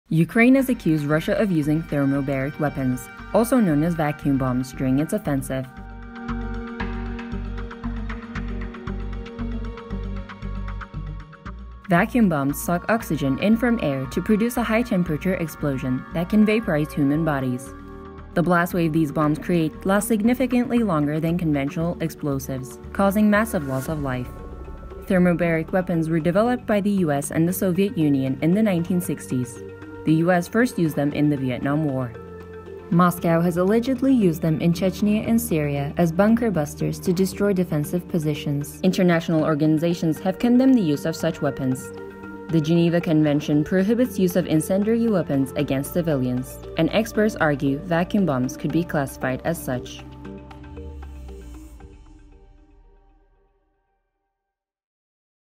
'Vacuum bombs' explained
Ukraine’s ambassador to the US, Oksana Markarova, accused Russia of using a thermobaric weapon known as a ‘vacuum bomb’ during its attack. Here’s what you need to know about it.